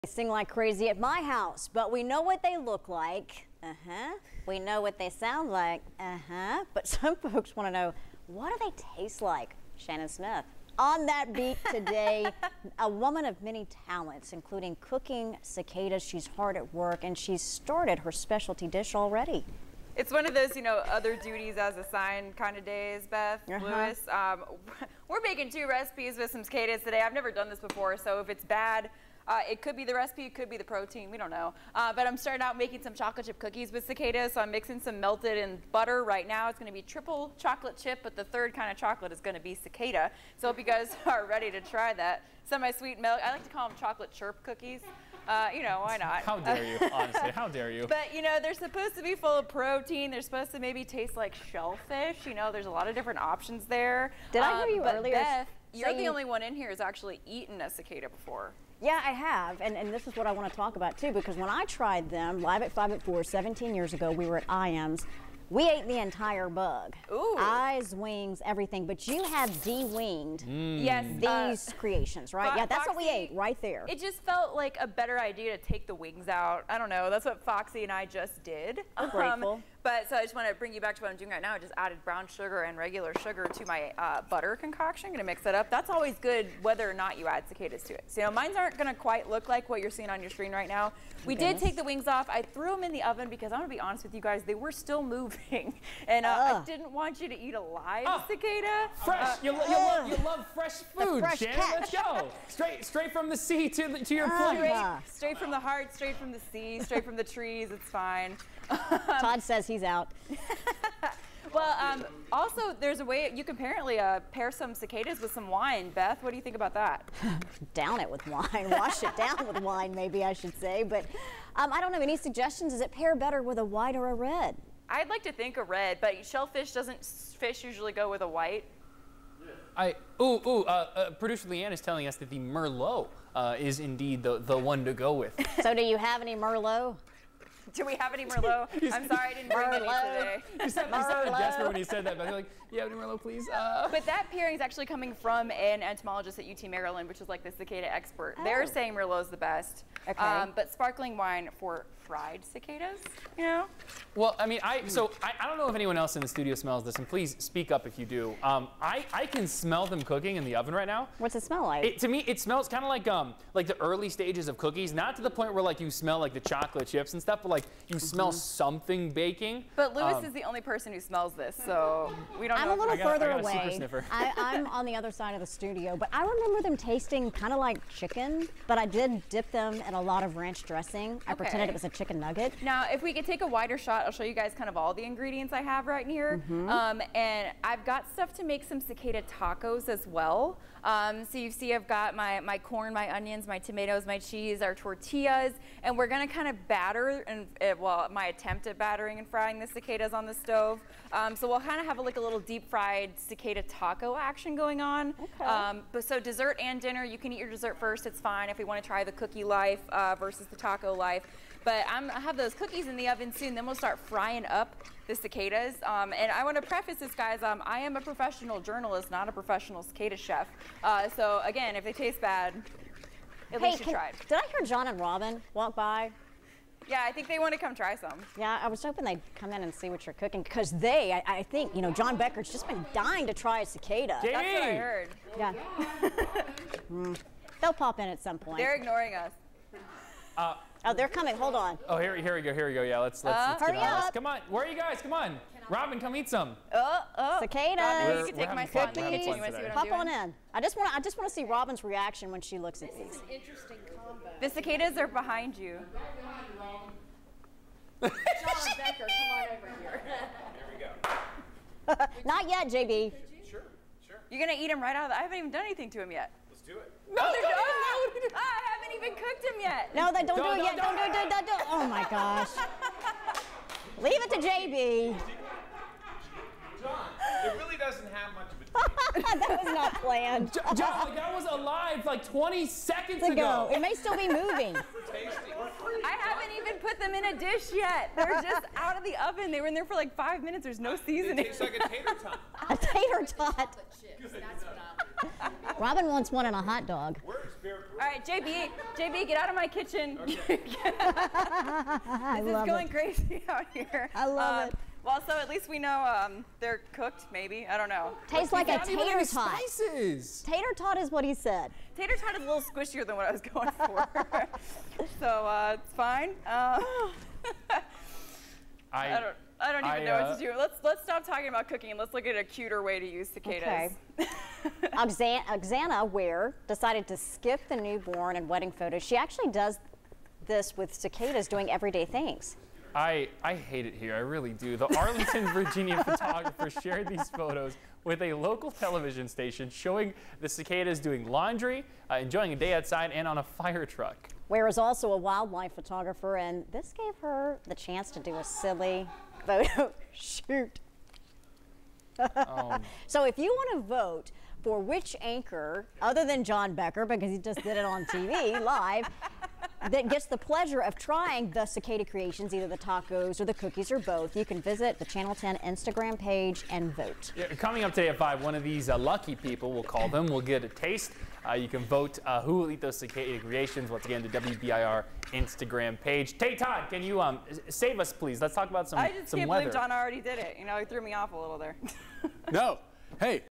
Cooking with cicadas
We know what cicadas look and sound like. But, how do they taste? John Becker couldn't wait to tr. May 24, 2021-4pm.